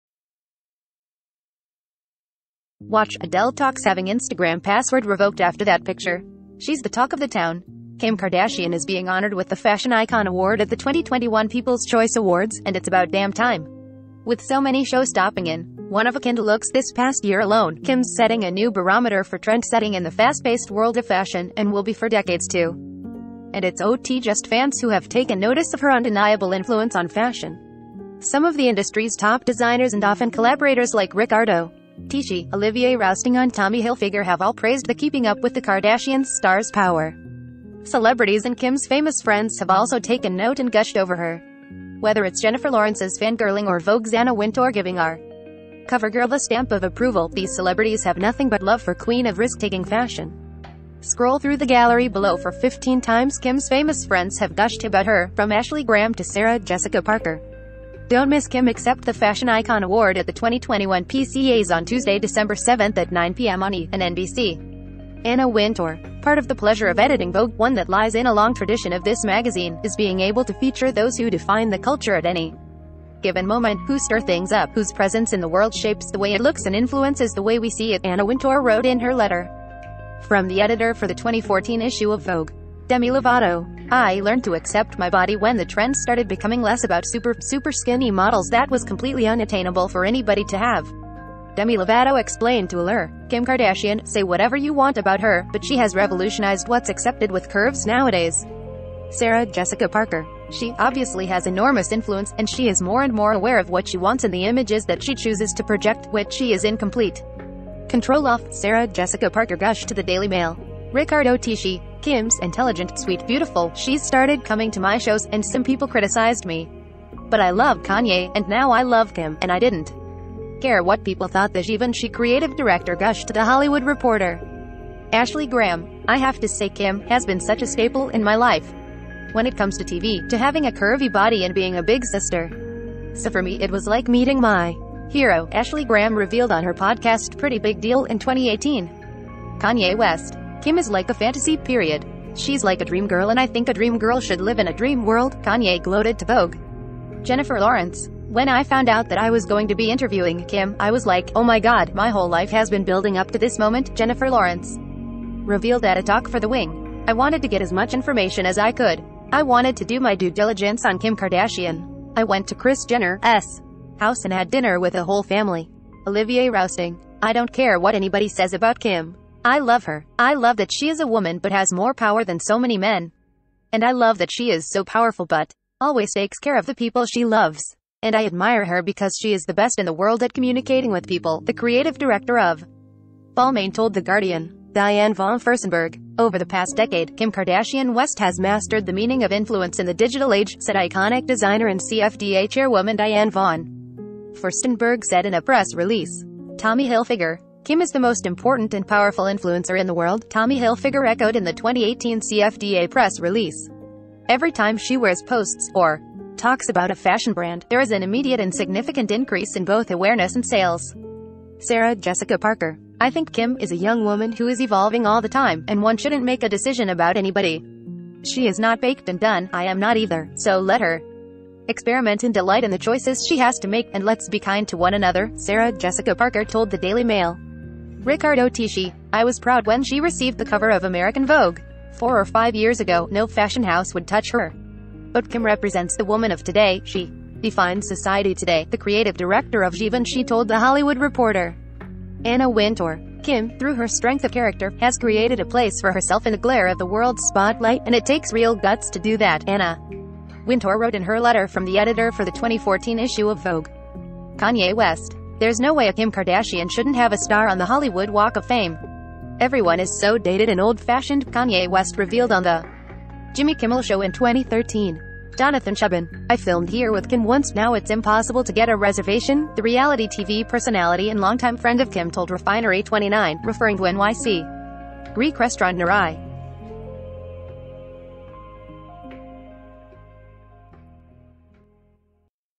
All the Celebs Who've Gushed Over Kim Kardashian's Style & Influence - E! Online
Watch: Adele Talks Having Instagram Password Revoked After THAT PictureShe's the talk of the town!Kim Kardashian is being honored with the Fashion Icon Award at the 2021 People's Choice Awards, and its about damn time! With so many show-stopping andone-of-a-kindlooks this past year alone, Kim's setting a new barometer for trendsetting in the fast-paced world of fashion, and will be for decades toAnd it's ot just fans who have taken notice of her undeniable influence on fashion. Some of the indu credit